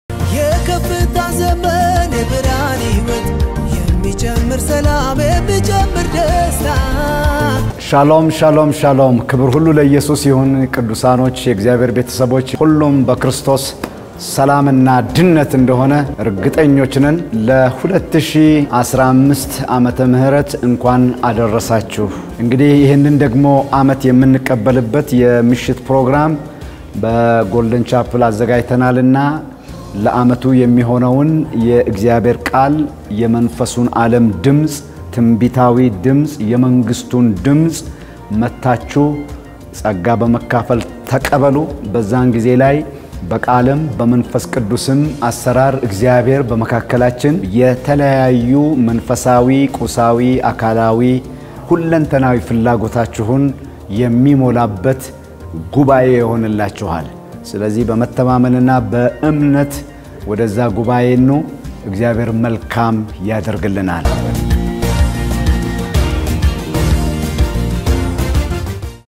እንኳን አደረሳችሁ ! ነገ አርብ ምሽት በበዓል ዋዜማ ልዩ ዝግጅት
Tomorrow night Friday at 11:00 (local time) we will have a wonderful time in worship with God's word and art. You are all invited